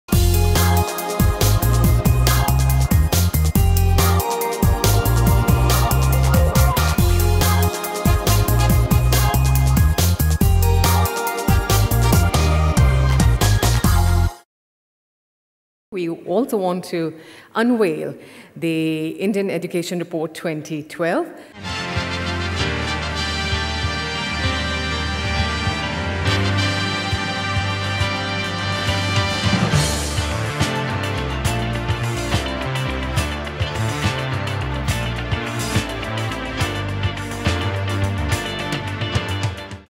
Indian Education Congress Award,2012 part-1
Indian Education Awards are the most prestigious awards that recognize excellence in the education sector. Awardees will receive national recognition and will be termed as best education entities in the country. The winners will be awarded a trophy or a plaque. The award winners will receive extensive media coverage and PR with the partners. Such an achievement & recognition have, according to our Award recipients, translated into further success in the education business.